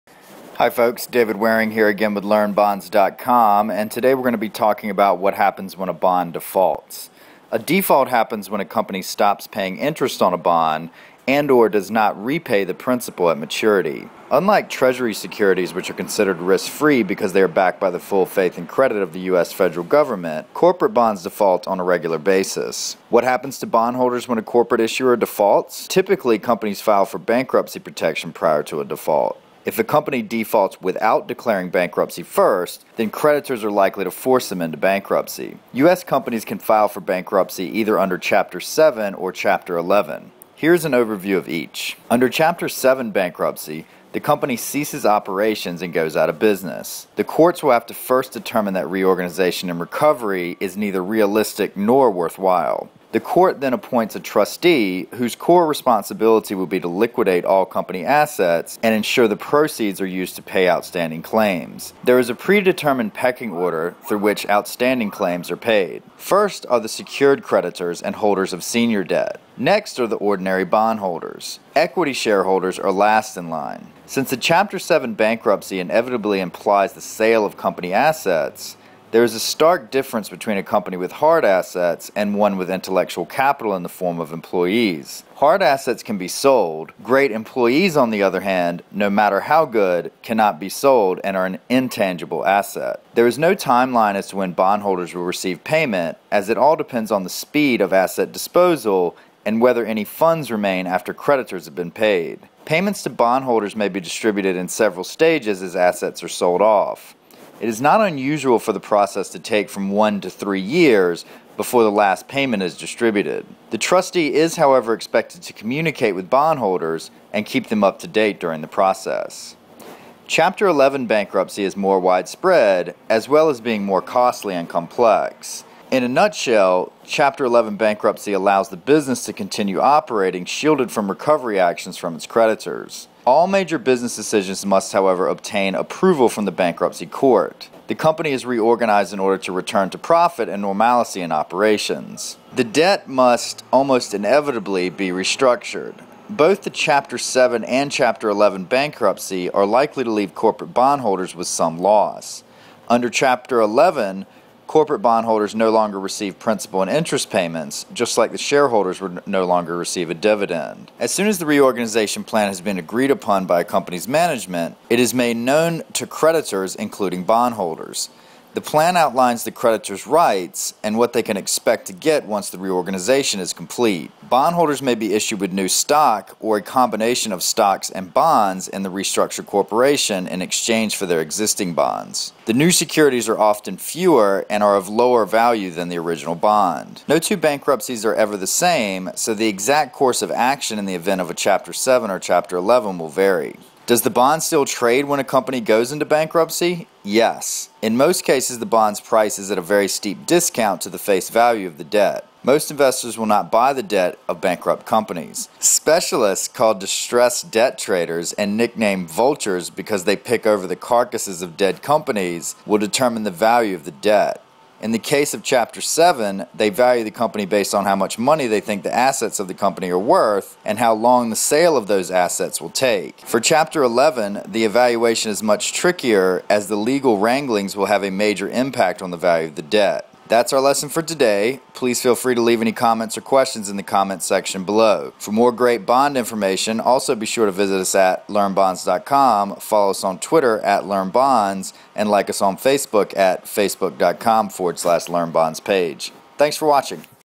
What Happens When a Bond Defaults
- We here about bond defaults all the time but what exactly happens when a corporate bond defaults?